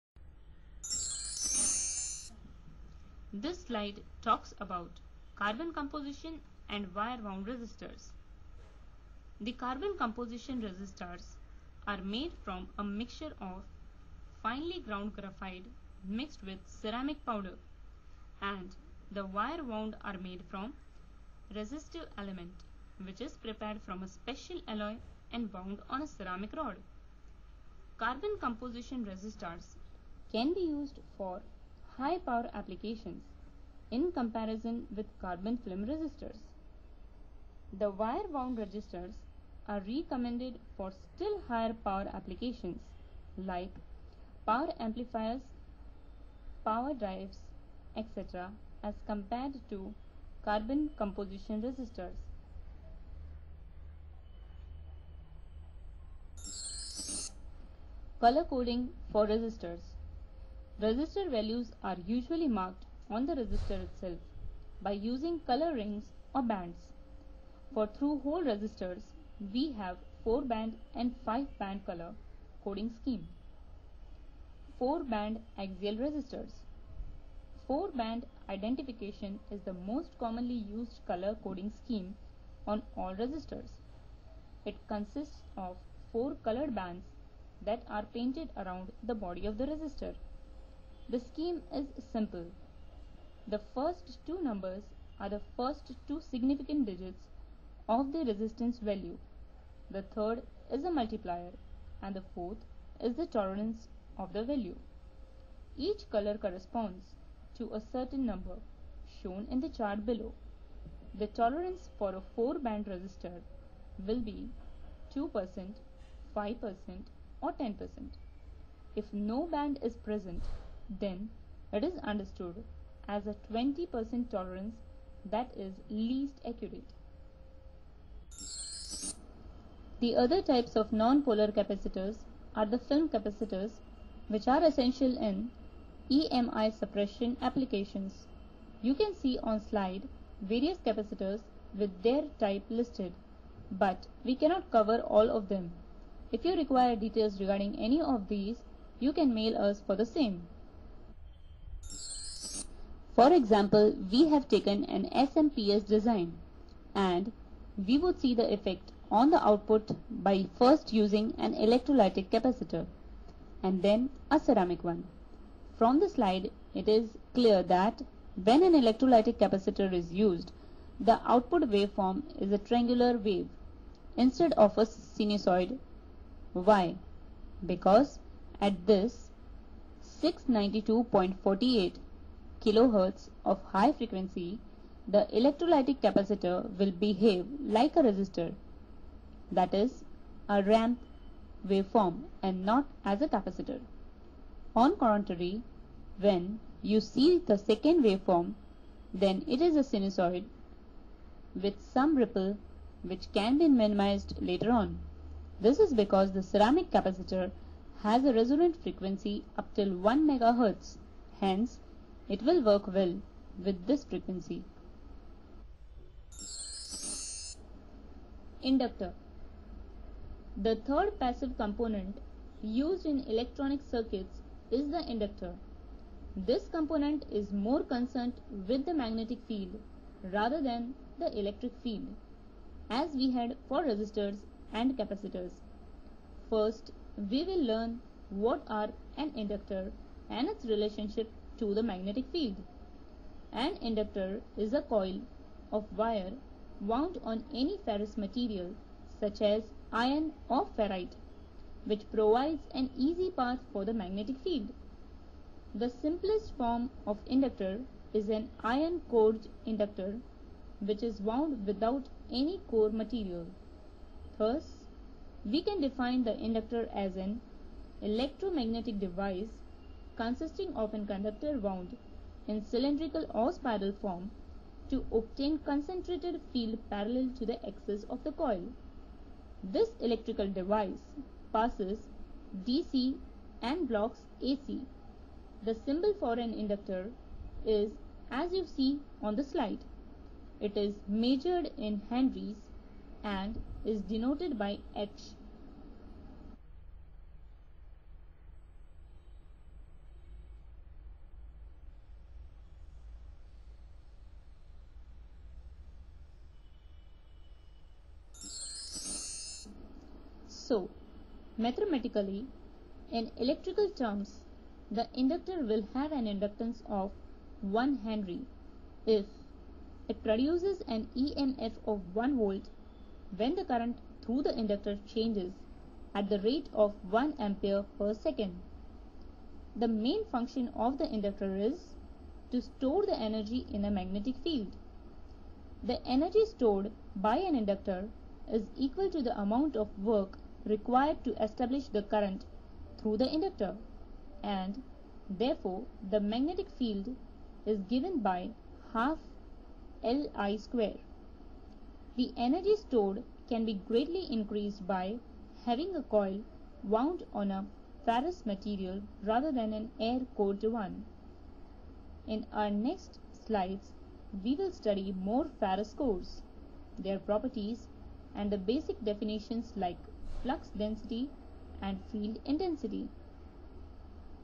Electronics Material & Components
This CD is made to explain the basics and different types of resistors, capacitors and inductors.Its features also.